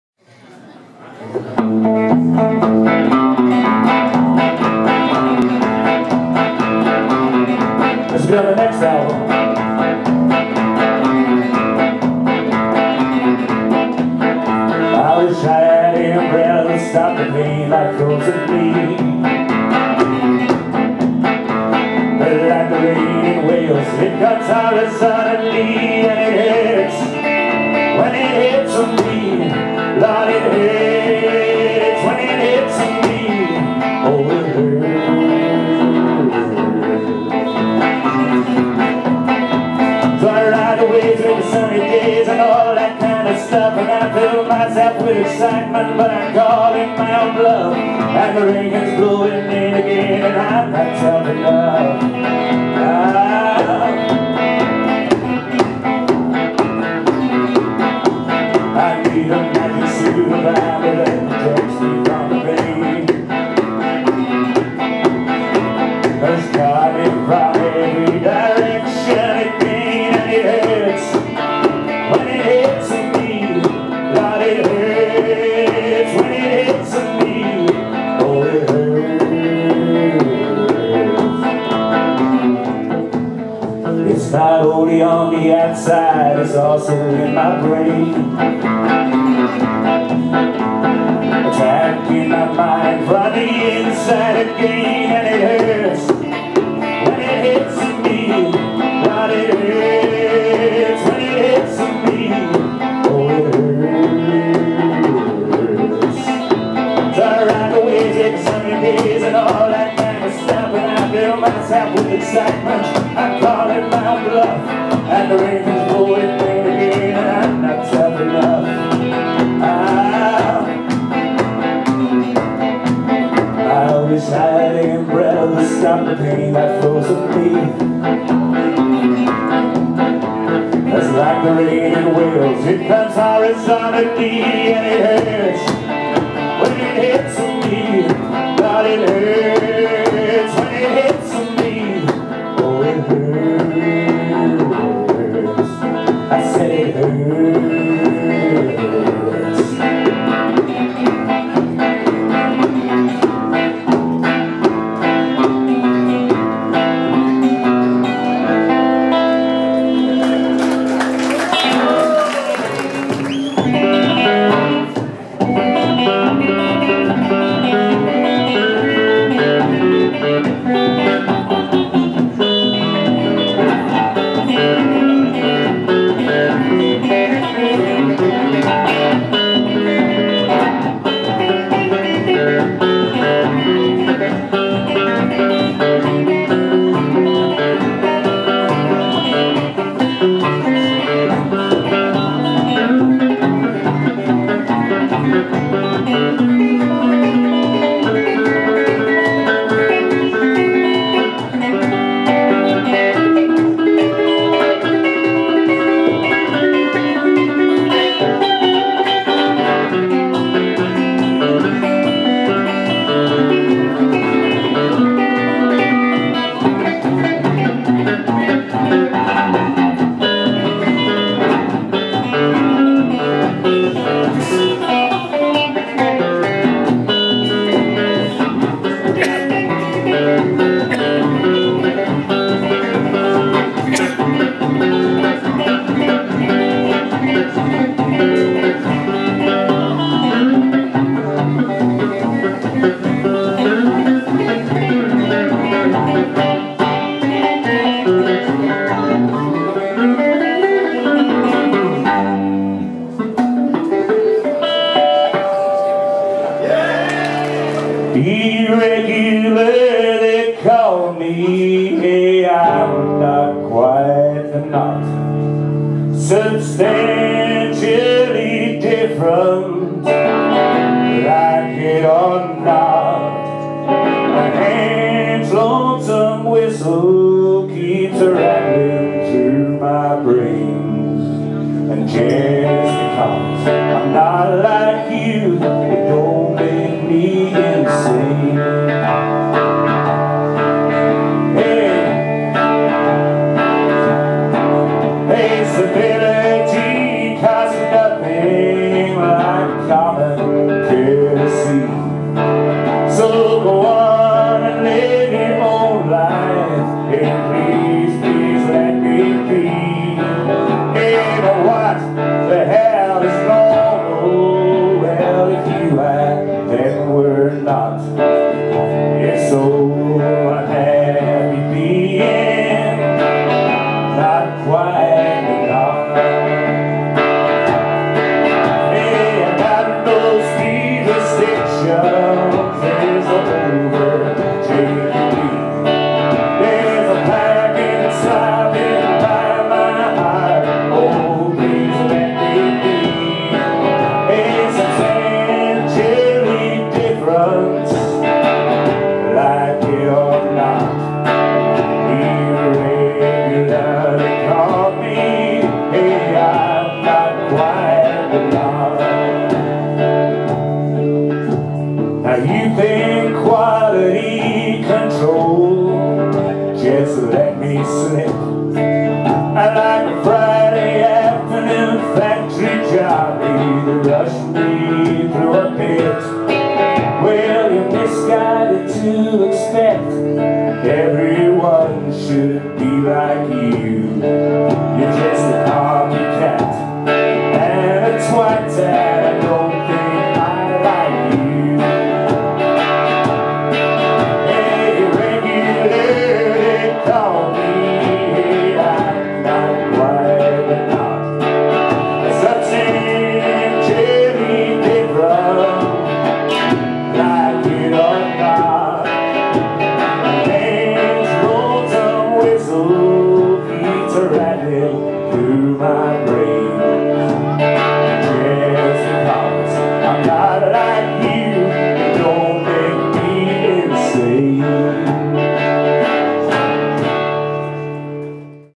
John Lewis, 100 Club, 11.5.13.
John Lewis performs solo at The 100 Club.